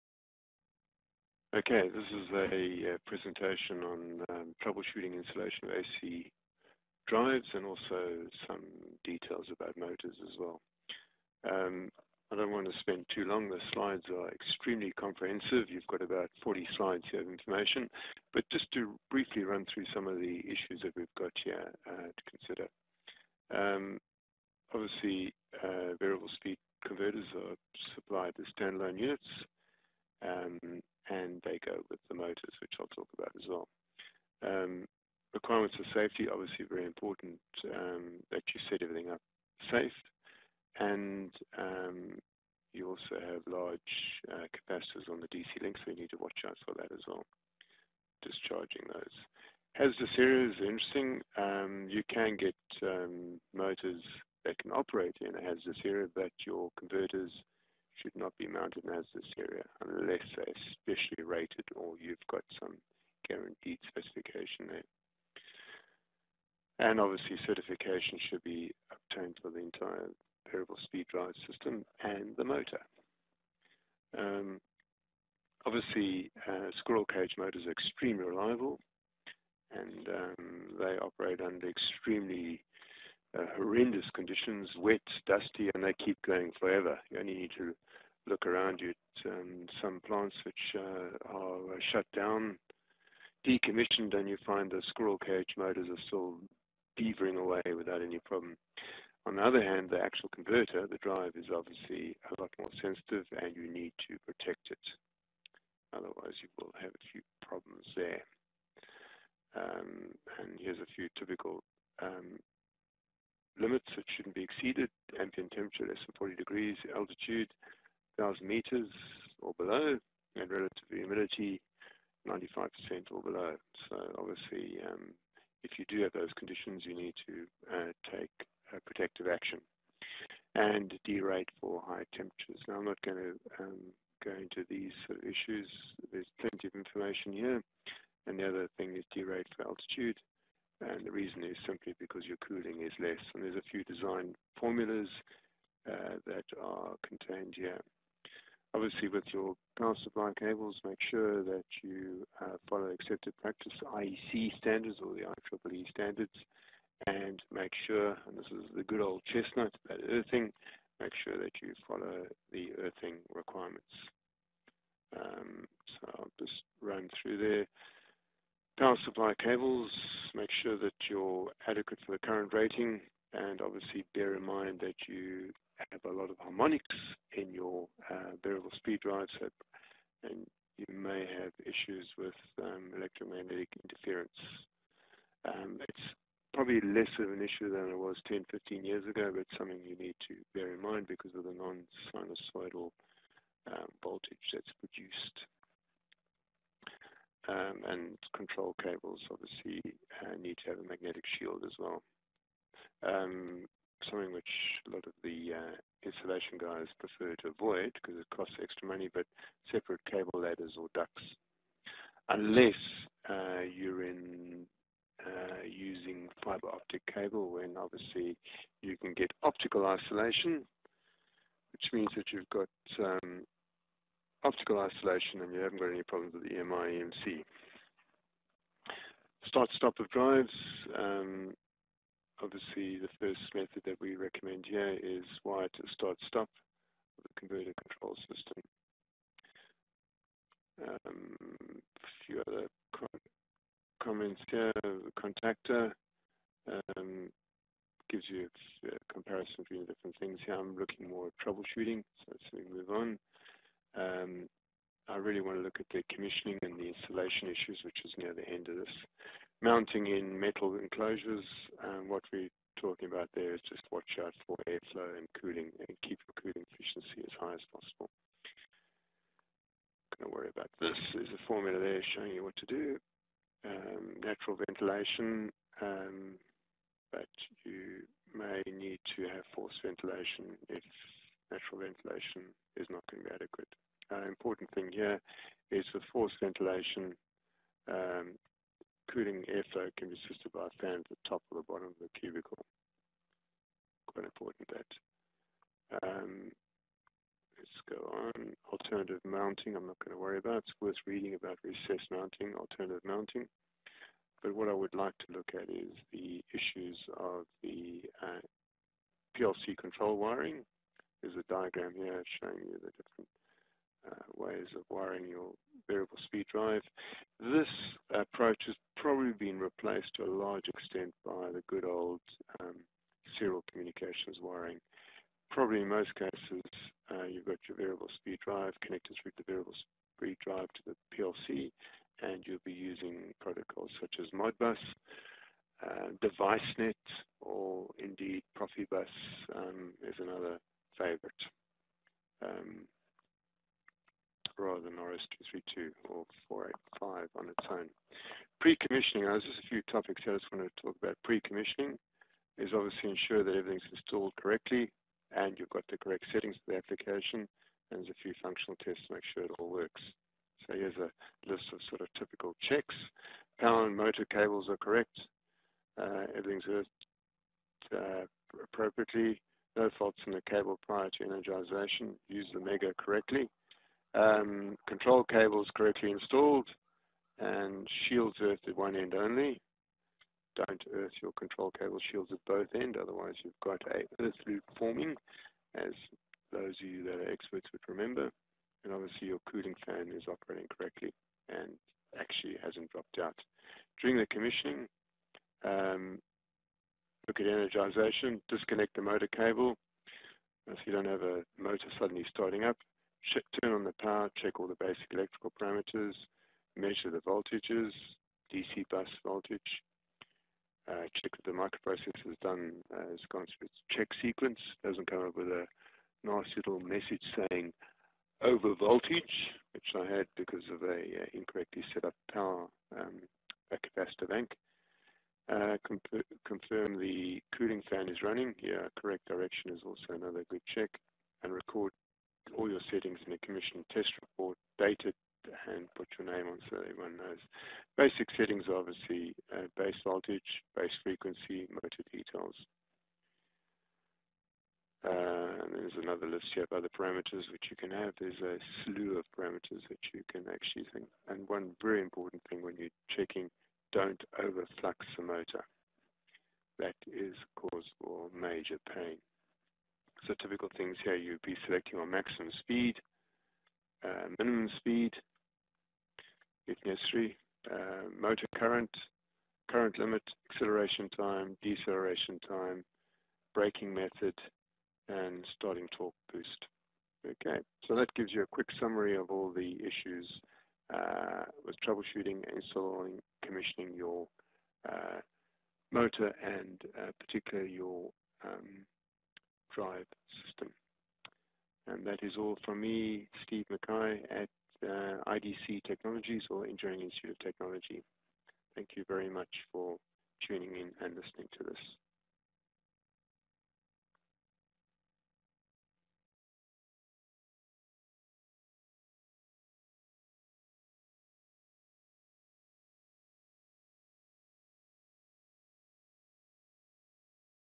Troubleshooting and installation of AC Drives
It is estimated that electrical drives and other rotating equipment consume about 50% of the total electrical energy consumed in the world today.  The cost of maintaining electrical motors can be a significant amount in the budget item of manufacturing and mining industries. This workshop gives you a thorough understanding of electrical motor’s working, maintenance and failure modes and gives you the tools to maintain and troubleshoot electrical motors.

You will gain a fundamental understanding of the installation, operation and troubleshooting of electric motors. Typical applications of electric motors in mining, manufacturing, materials handling, process control are covered in detail. You will learn the basic steps in specifying, installing, wiring and commissioning motors. The concluding section of the workshop gives you the fundamental tools in troubleshooting motors confidently and effectively.